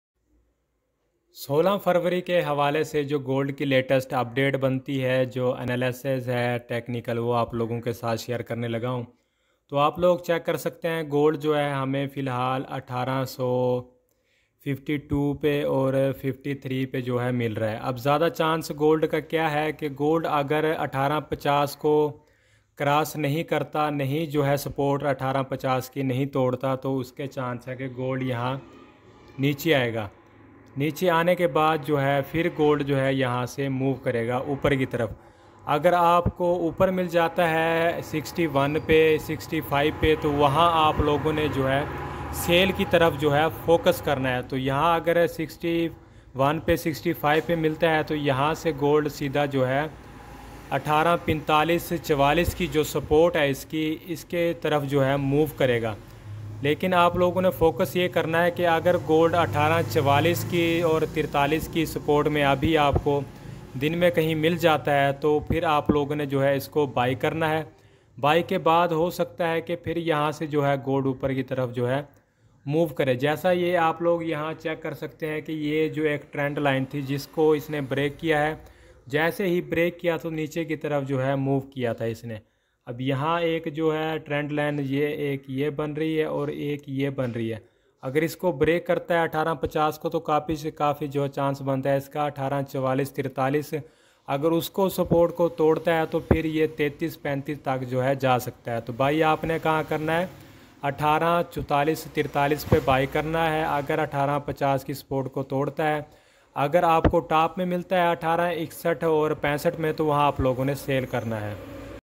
Technical Analysis Of Gold 16th February 2022 | Today Gold Best Trading Strategy
Technical  analysis  of gold 16th February. In this video we will show you gold best trading strategy which help you to make good profit  a big investment  a big profit. Always use proper money management. If you have many loss in trading  then we help you to recover your losses

Follow Us Free Signal!